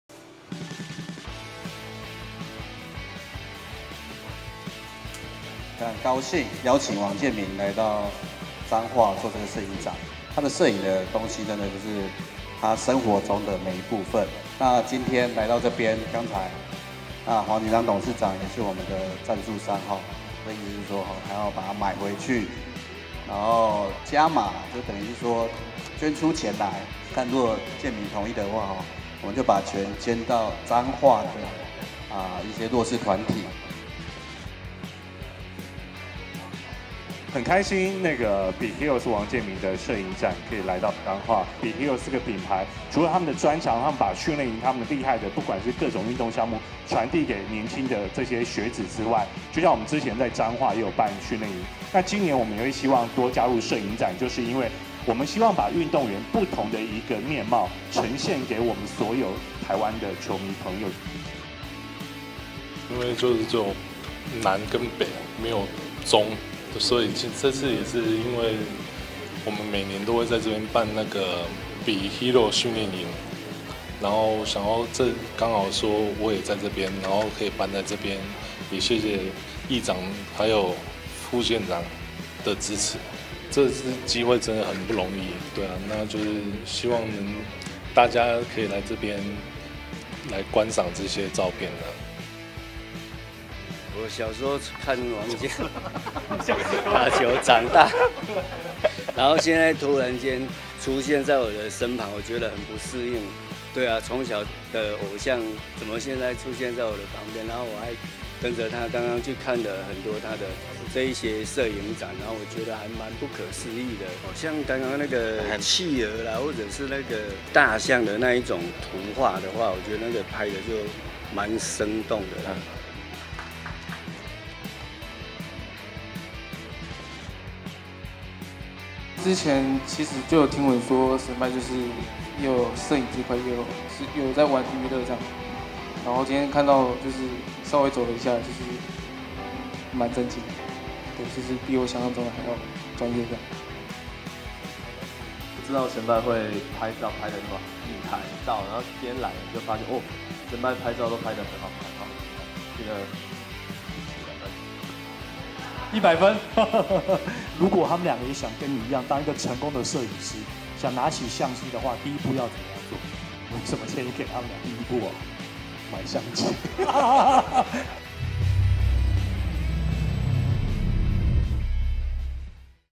《棒球》王建民攝影展移師彰化 BC台灣柏釧做公益
「沉默的王牌」王建民今年先後在臺南與臺北舉辦「WANG’S LIFE王建民攝影展」，吸引眾多球迷關注，並一同前往欣賞建仔透過相片記錄生活的感動，獲得大量好評與回響，更引來中部球迷「敲碗」，為了回應中部球迷的熱情期待，建仔特地於12月17日加開中部場，將在彰化縣議會新會館展出，一路展到12月30日，期待球迷把握機會，免費入場參觀，一起來欣賞建仔用相片說故事。

說到接觸攝影的開始，王建民表示，是因為旅美期間認識不少媒體攝影記者，因此對攝影開始產生興趣，2009年因為大兒子的出生，想為自己和家人留下記錄，讓他開始拿起相機，捕捉每一個生活瞬間，用鏡頭訴說生活中的故事，讓自己從影中人變成記錄者。王建民與旅美小將林暉盛、中信兄弟林志綱一同出席攝影展開幕式，三人展開運動員對談，聊到許多旅美趣事，攝影展中也有許多建仔的旅美記憶，出身彰化的林志綱，則提供幾個彰化景點，還希望前輩能到自己的母校彰化藝術高中走走。

BC台灣柏釧董事長黃錦章長年關注體育發展，作為在地企業，他大力支持BE HEROES系列活動。熱愛體育的他，從王建民的球迷，變成攝影作品的粉絲，他公開宣布，將購買王建民的攝影作品，捐助弱勢團體，議長謝典林強調，疫情期間公益團體的收入驟降，感謝黃董的善心義舉。除了發善心，黃錦章期待透過更多體育活動的推廣，能進一步鼓勵更多年輕朋友參與運動，甚至學習王建民的精神，勇敢挑戰夢想。